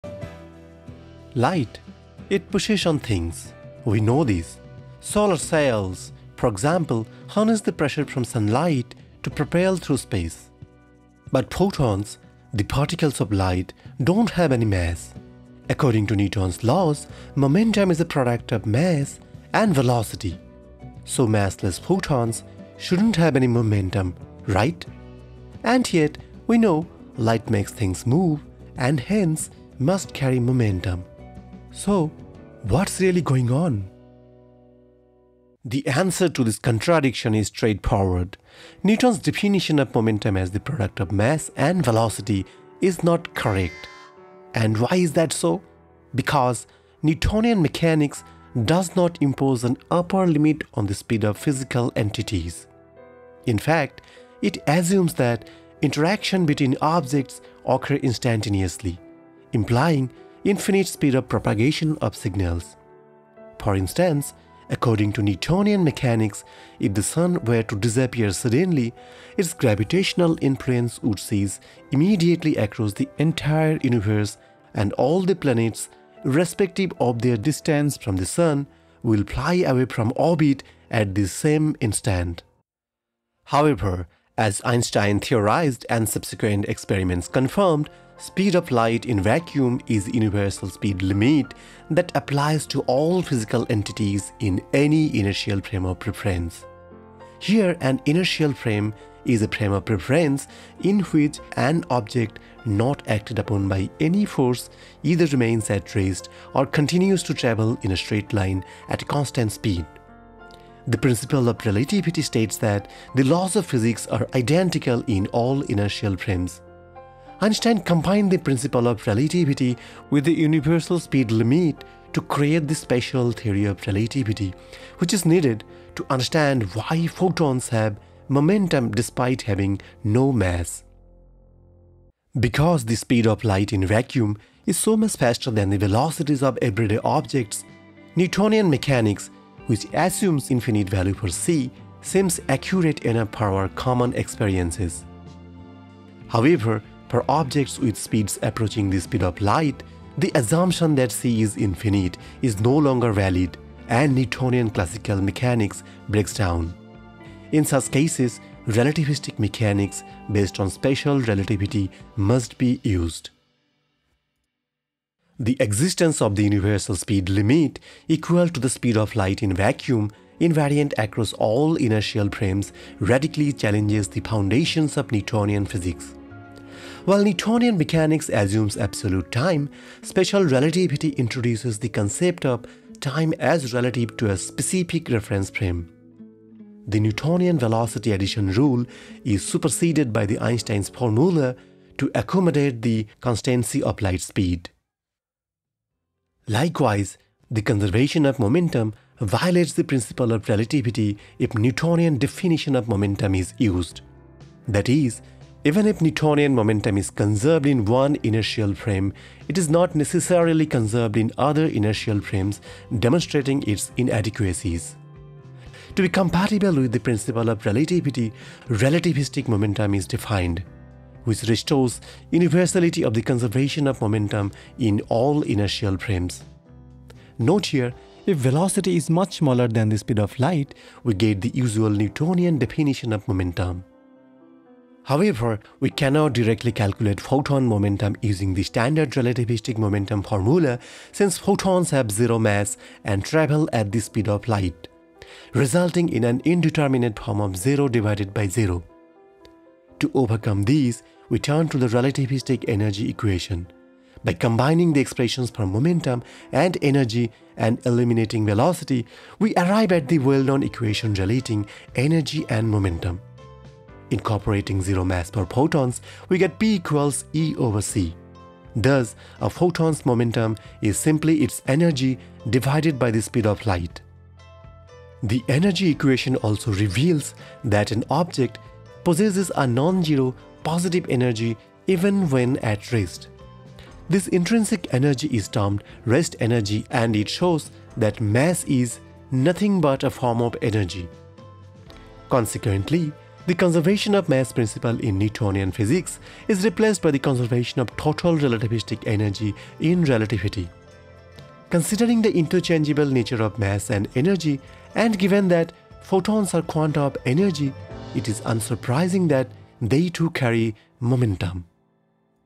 But why photons have momentum despite having no mass?
The definition of momentum used in Newton’s laws of motion is NOT useful for photons! Find out where Newton was wrong and how Einstein remedied it!
N.B: The trick that special relativity uses is to use proper velocity in the definition of momentum. Proper velocity is simply distance travelled divided by the proper time i.e. the time elapsed in a clock moving with the object.

References (Books):
1. “The Classical Theory of Fields”, L. D. Landau, E. M. Lifshitz.
2. “Classical Electrodynamics”, J. D. Jackson.
3. “Introduction to Electrodynamics”, D. J. Griffiths.

Credits:
Math equations created using the open source library Manim, maintained by Manim Community.

Chapters:
0:00 Intro
0:35 Newton’s mistake
1:22 Einstein’s correction
3:15 Momentum and Relativity
4:35 Final words